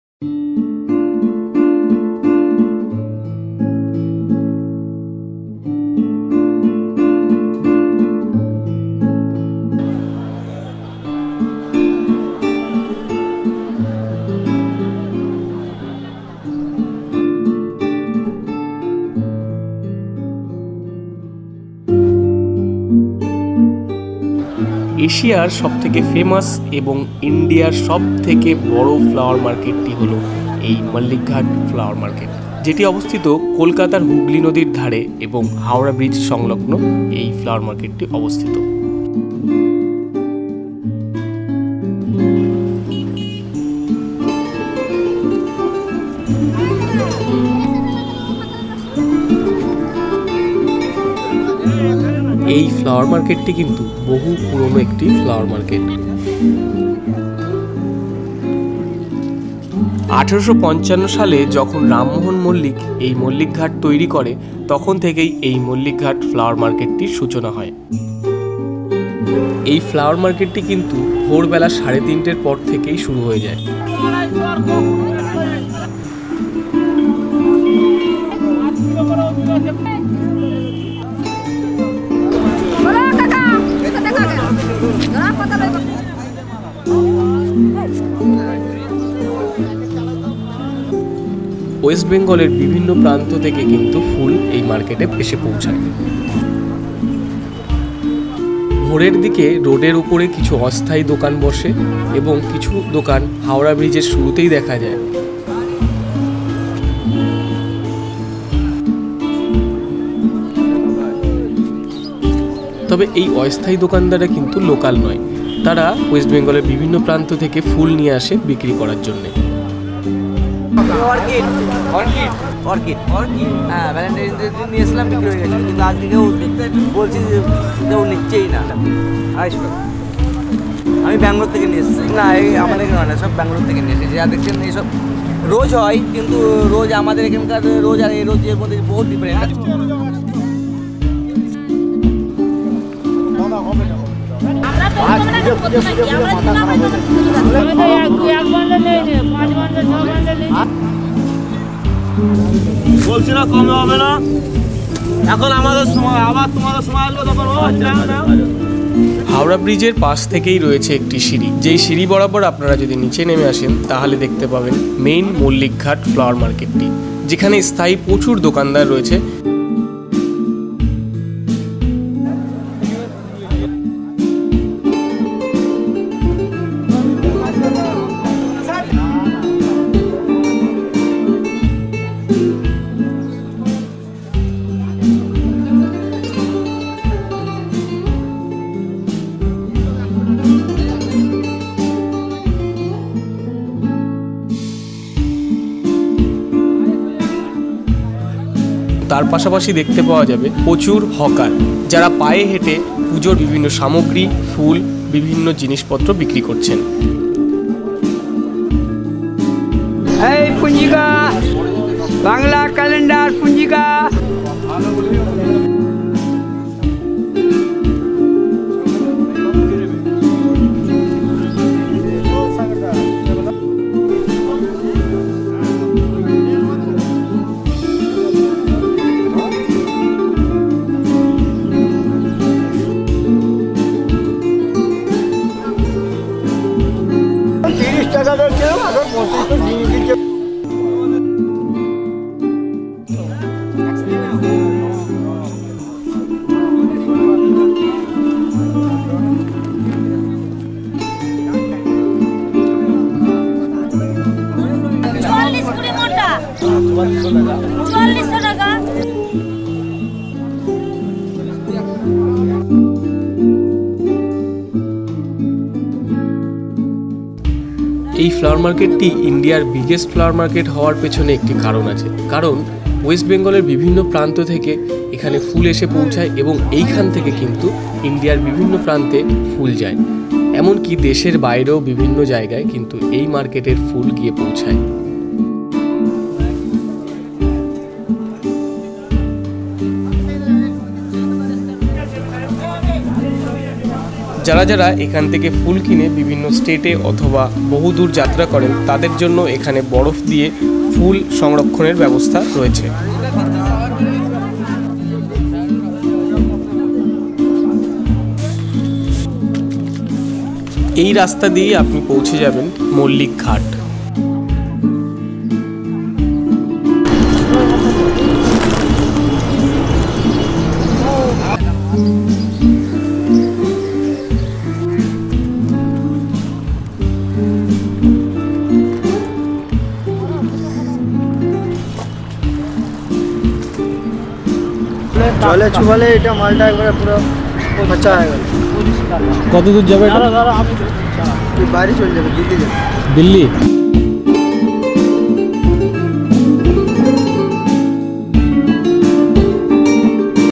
Mullick Ghat flower market | Biggest flower market in India
This video is about Famous as the biggest flower market in Asia, the Mullick Ghat, Kolkata, West Bengal, INDIA.
My equipment:
Rode Video Mic Go
Rode VideoMicro
My Lens
My Camera
My Gopro
My tripod cum monopod
My sony Card